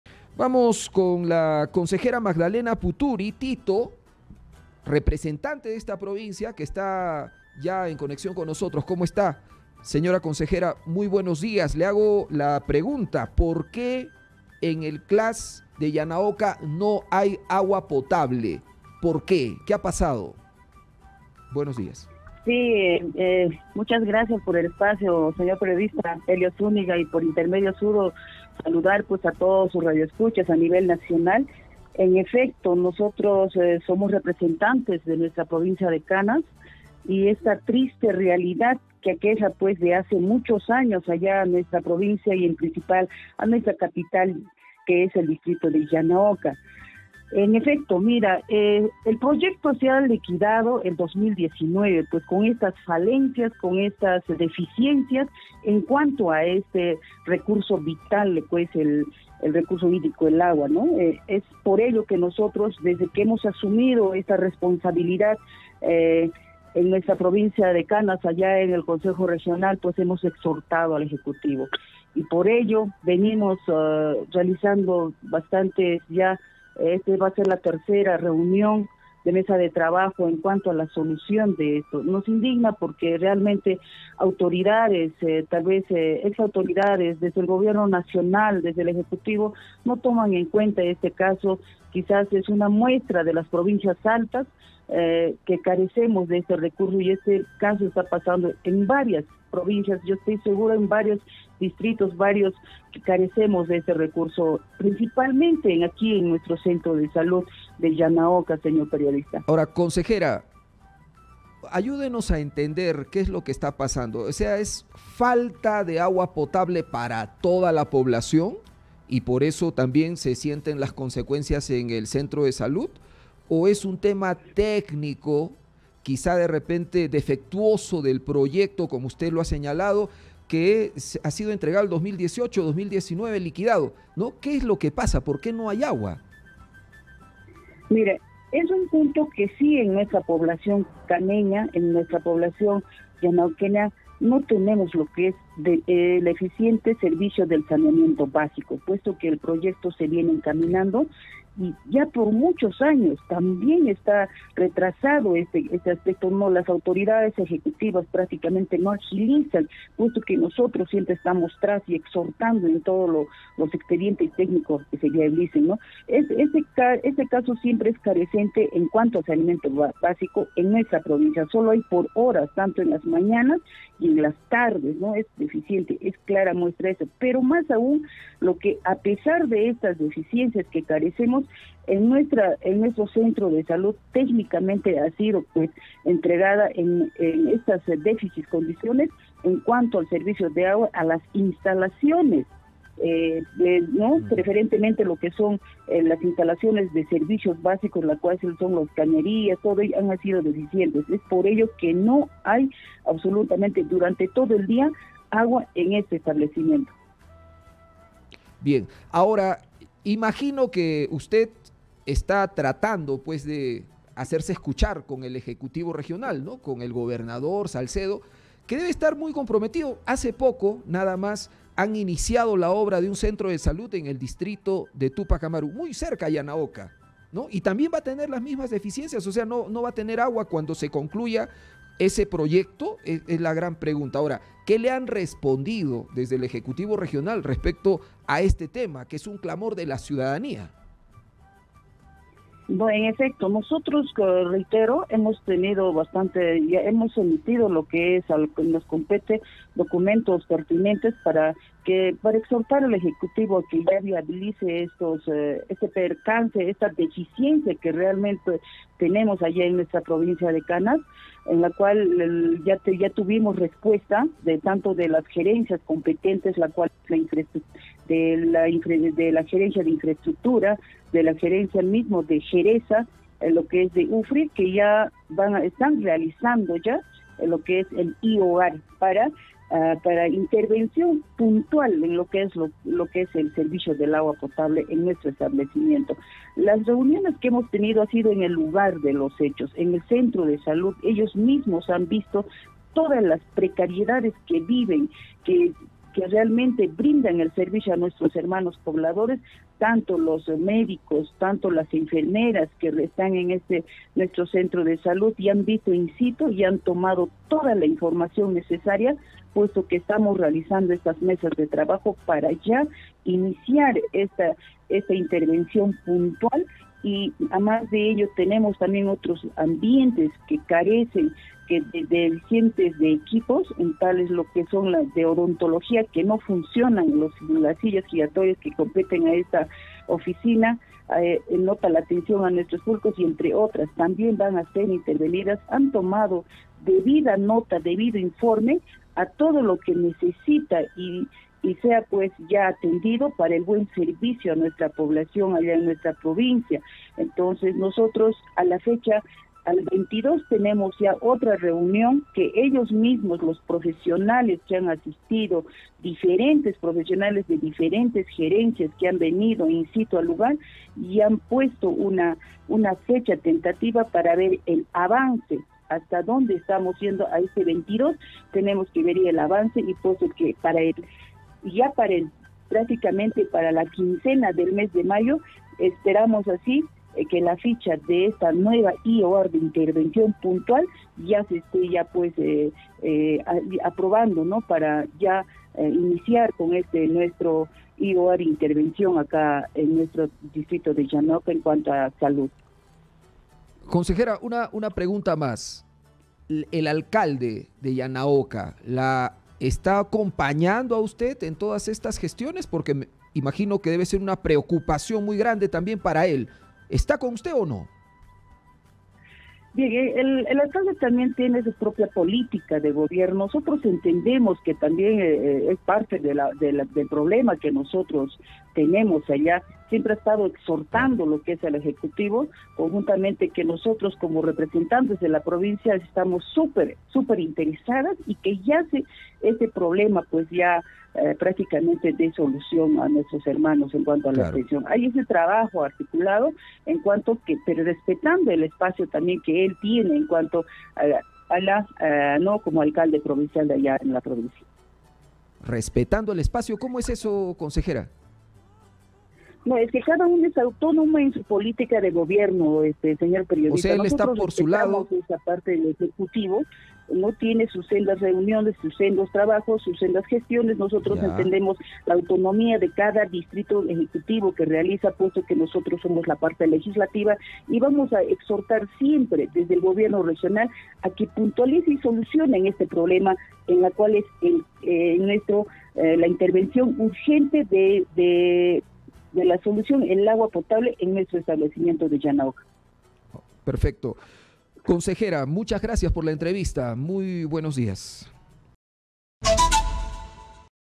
🔴🔵Consejera regional exige al Gobierno finalizar obras para que Yanaoca tenga acceso al agua
En diálogo con Exitosa, la consejera regional de Canas, Magdalena Phuturi Ttito, se refirió a la falta de agua en un centro de de salud de Yanaoca y señaló que han exhortado al Gobierno a que el vital recurso llegue a dicha provincia.

Exitosa Perú
"Exitosa: La voz que integra al Perú"

Corporación Universal